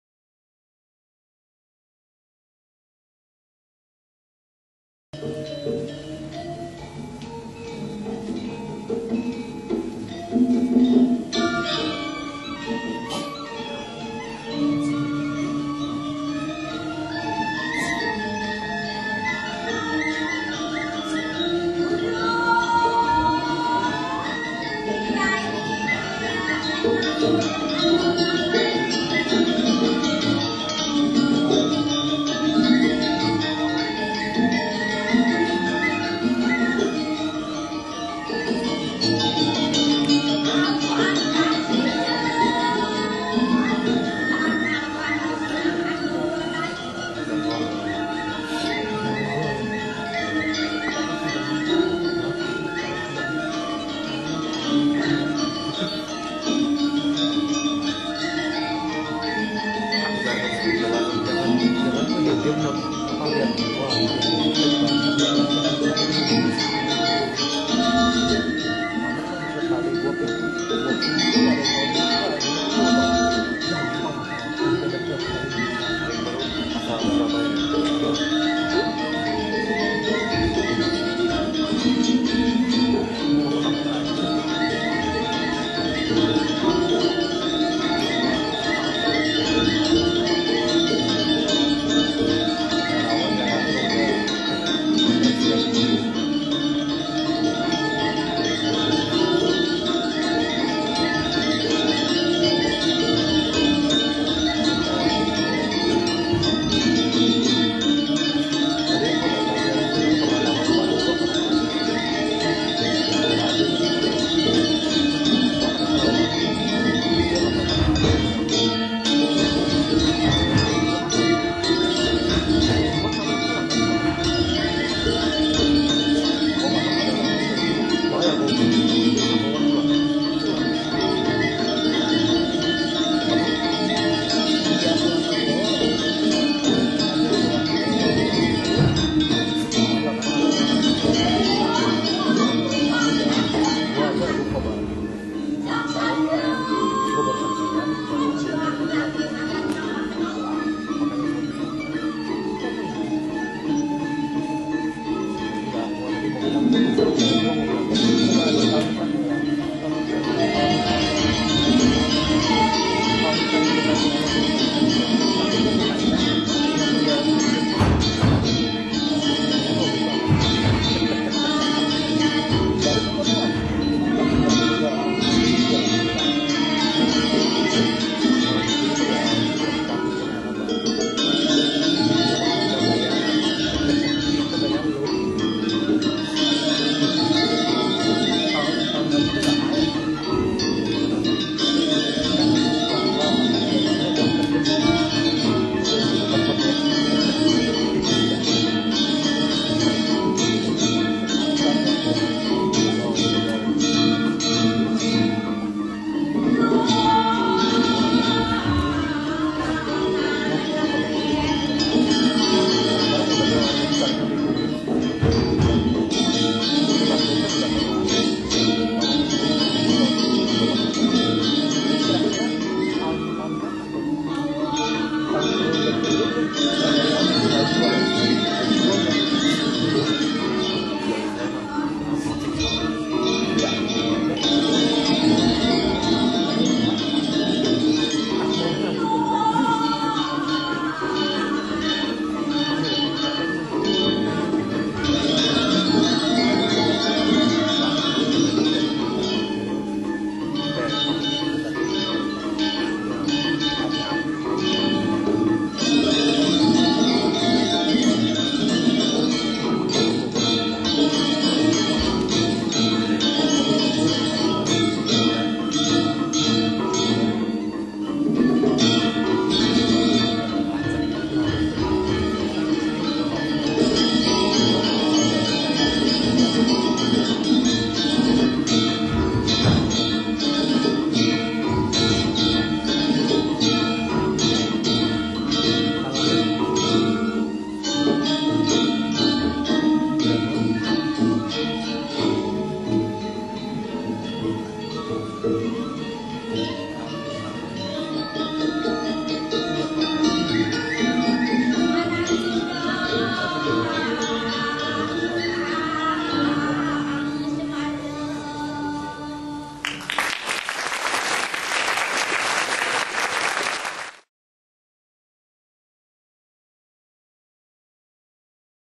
IUB, Indonesian Night 2007, Lengong Kraton Dance Part Two
Indiana University Bloomington Indonesian Student Association, Permias, presents teh Lengong Kraton Dance as performed by Sagung Mirah and Sagung Chika Kertayuda from the Indonesian Performing Arts of Chicago.  The dance is one of Bali's most famous.  It is a sacred dance of nymphs;  dedari, who tell the story of the ill-fated courtship of Lasem.